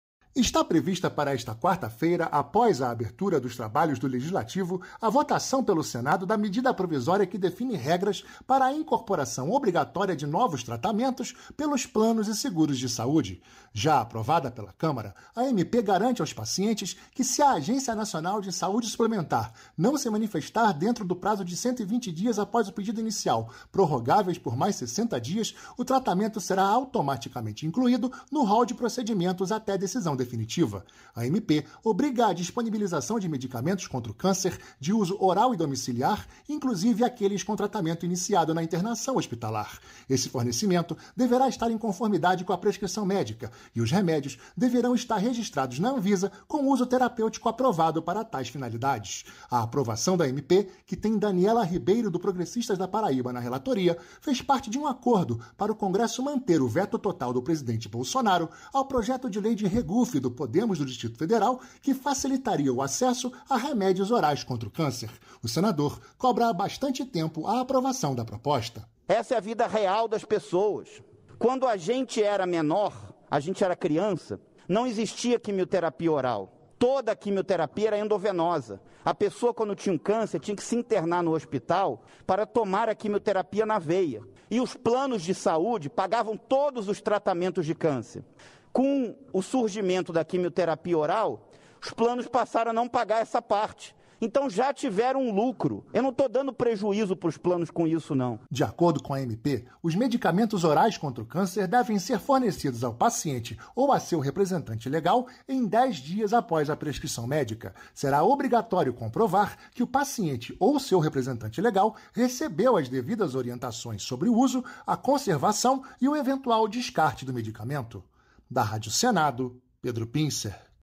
O SENADO FEDERAL VOTA MEDIDA PROVISÓRIA EM QUE PLANOS DE SAÚDE COBREM TRATAMENTO ORAL CONTRA CÂNCER
O SENADO FEDERAL VOTA NESTA QUARTA-FEIRA MEDIDA PROVISÓRIA QUE OBRIGA PLANOS DE SAÚDE A COBRIREM TRATAMENTO ORAL CONTRA CÂNCER.